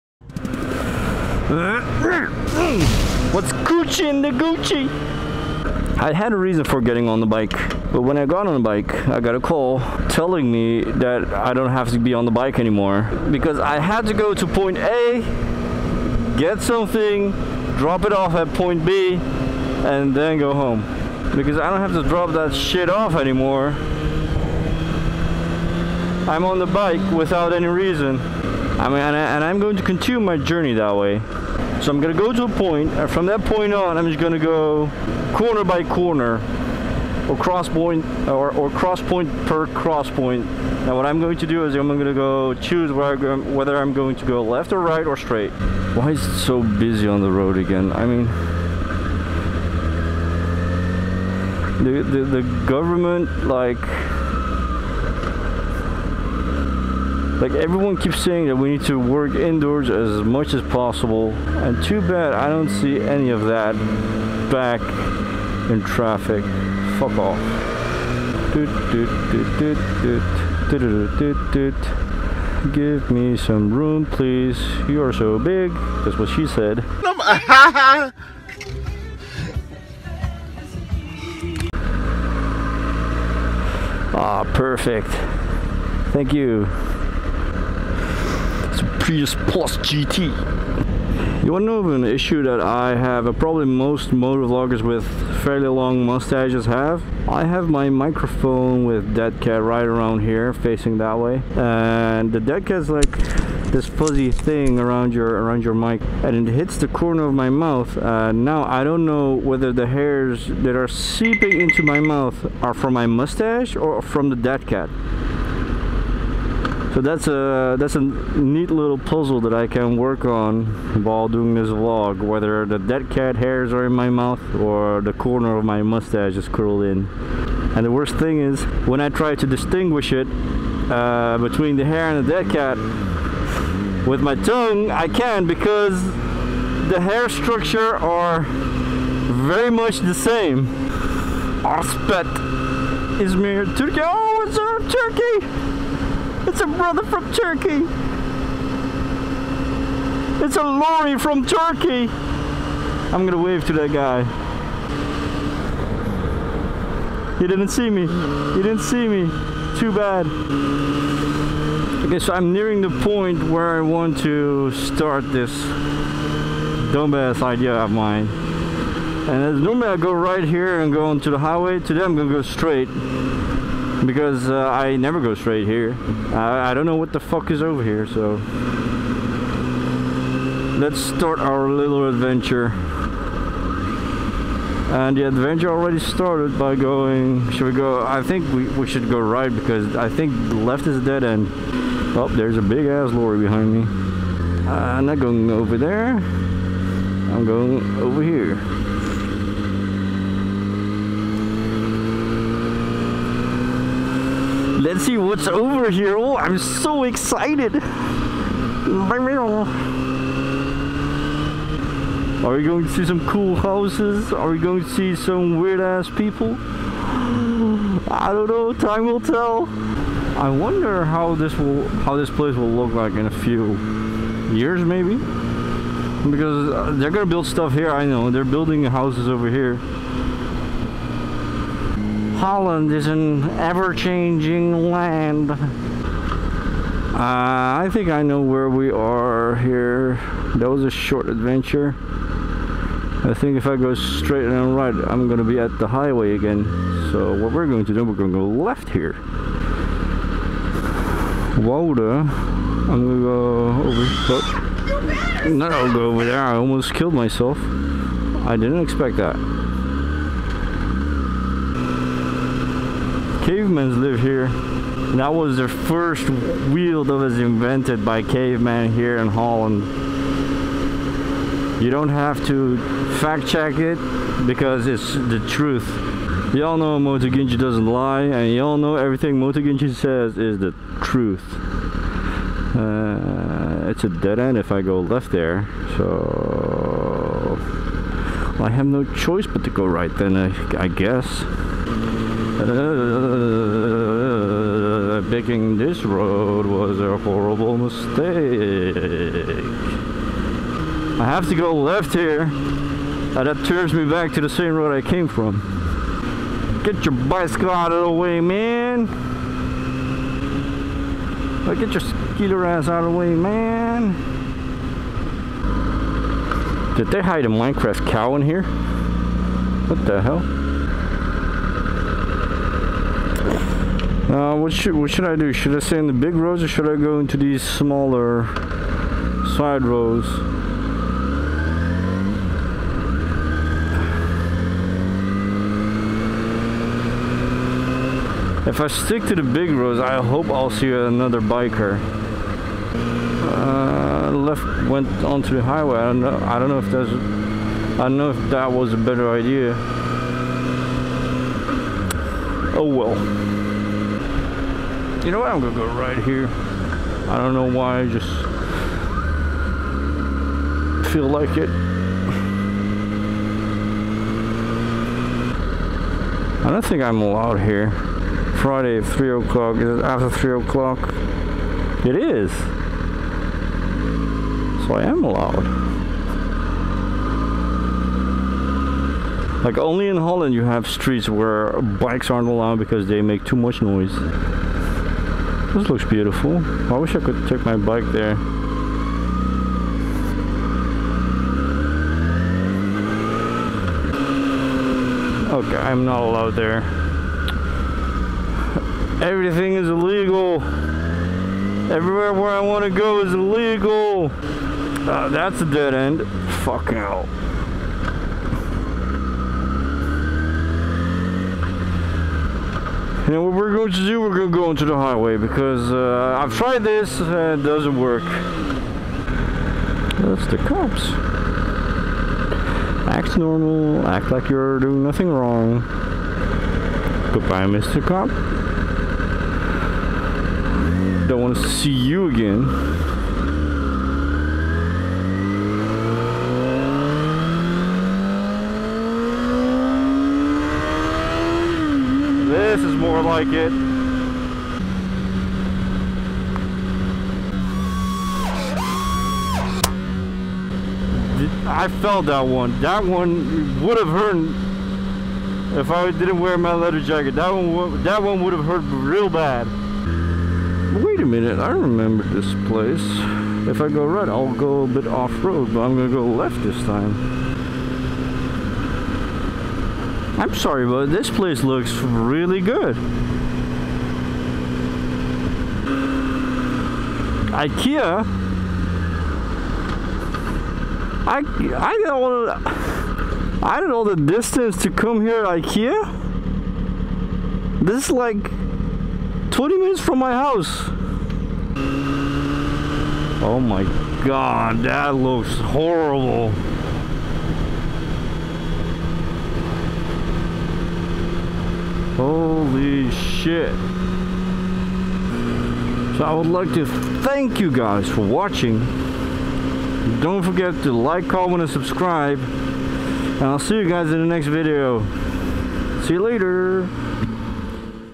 Ride your bike without a cause! Ride with MotoGinji! Get on your motorcycle and let's just ride!
In Today's motovlog, I just get on the bike and ride without a plan, destination or reason. I ride the bike without a cause! We take things crosspoint per crosspoint to see where we will go, where we ill end up and what we will do.

This was a stupid idea to do though for a motovlog XD